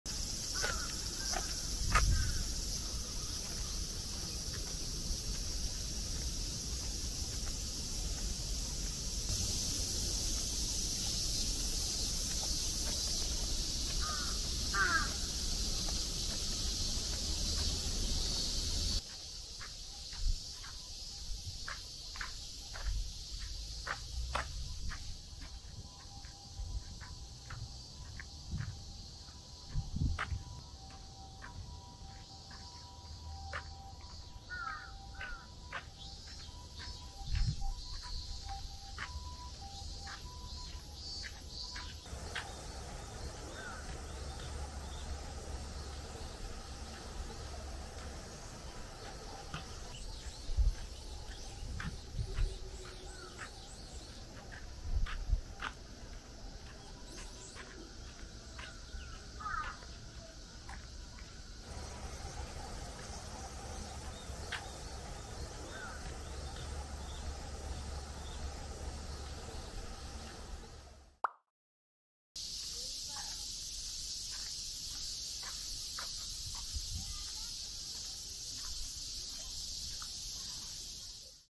【ノーブラ】近所の公園に散歩しに行ったら地元の中学生に...
近所の公園だからって油断してました😅
実はみんなの視線がどこにあるかってわかっちゃうんですよ👀
良い子はさり気なくしてくださいね😵